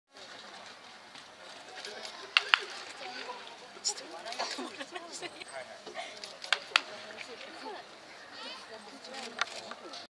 2014【ナゴヤ球場 秋季練習】浅尾拓也左打ち練習☆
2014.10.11　ナゴヤ球場　秋季練習
浅尾拓也投手、慣れない左打ちでフラフラしつつも打球は大きく飛んで行ってました。